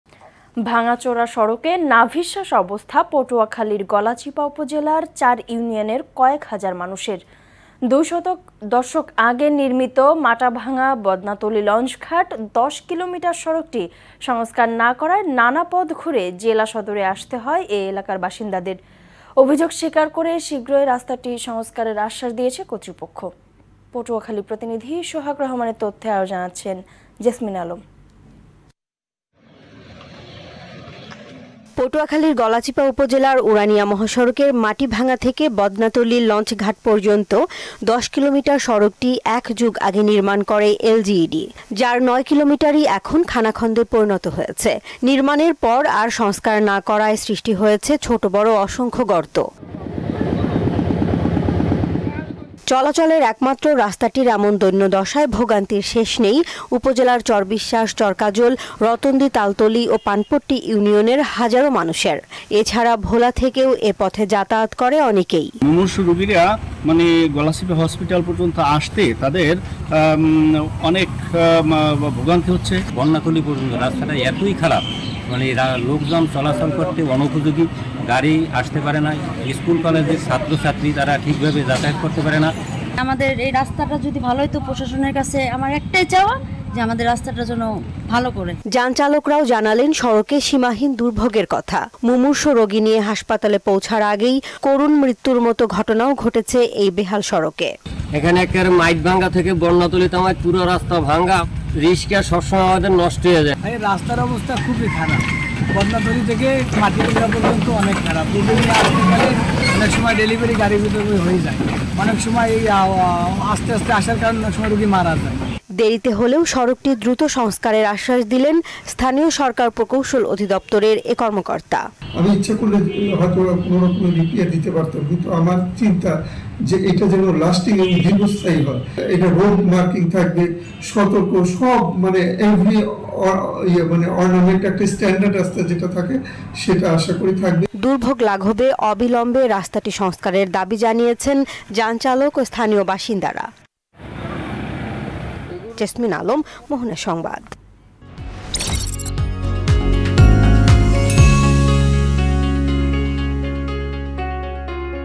পটুয়াখালীতে সংস্কারের অভাবে বেহাল মাটাভাঙ্গা বদনাতলী লঞ্চঘাট সড়ক | Patuakhali | Mohona Tv
ভাঙাচোরা সড়কে নাভিশ্বাস অবস্থা পটুয়াখালীর গলাচিপা উপজেলার চার ইউনিয়নের কয়েক হাজার মানুষের। দুই দশক আগে নির্মিত মাটাভাঙ্গা- বদনাতলী লঞ্চঘাট ১০ কিলোমিটার সড়কটি সংস্কার না করায় নানা পথ ঘুরে জেলা সদরে আসতে হয় এ এলাকার বাসিন্দাদের। অভিযোগ স্বীকার করে শিগগিরই রাস্তাটি সংস্কারের আশ্বাস দিয়েছে কর্তৃপক্ষ।